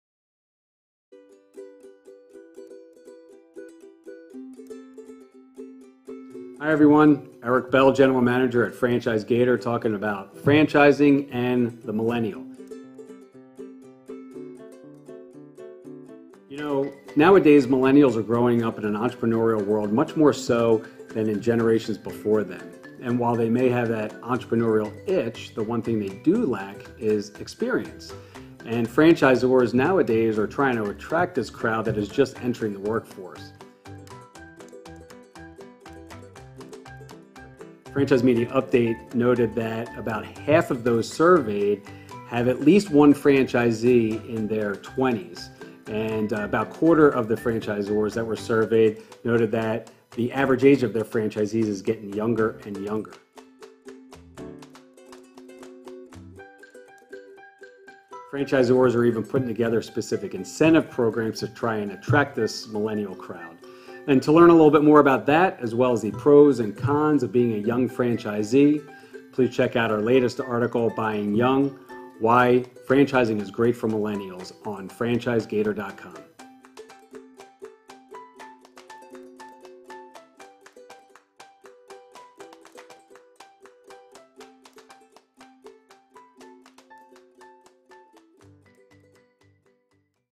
Why Franchising is Great for Millennials | Franchise Gator
Franchising offers a range of opportunities to fit the interests and skills of Millennials looking to start a business, many being low-cost and home-based businesses that offer both the flexibility and independence they seek. Read the full article on the Franchise Gator blog. goo.gl/UtspIh

Check out the hundreds of opportunities on Franchise Gator's extensive franchise directory